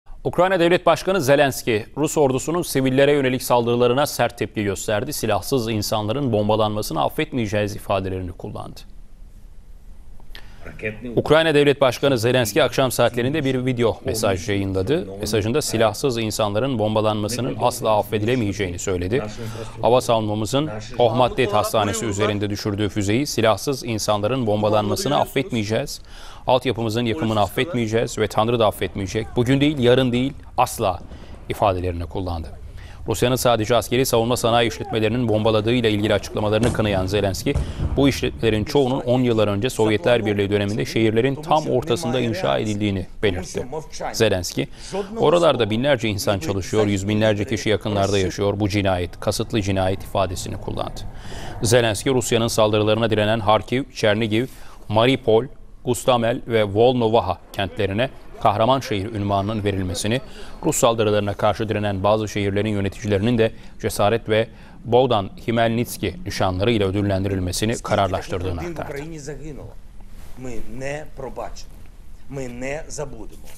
Ukrayna Devlet Başkanı Volodimir Zelenski Video Mesaj Yayınladı
Ukrayna Devlet Başkanı Volodimir Zelenski, pazar akşamı yayımladığı video mesajında tüm dünyaya seslendi.